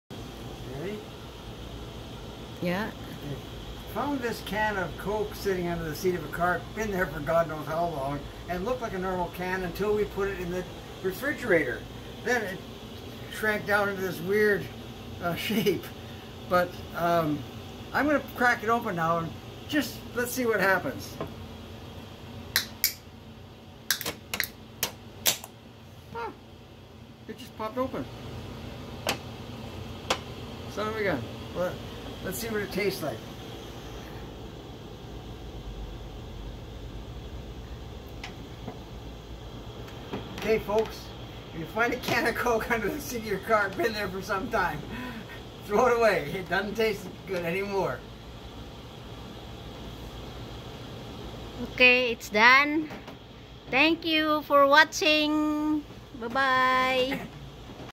FOUND A CAN OF COKE SITTING UNDER THE SEAT OF THE CAR BEEN THERE WHO KNOWS FOR HOW LONG.
If you find a can of soft drink under the seat of your car been there some times. Throw away it doesn't taste good anymore.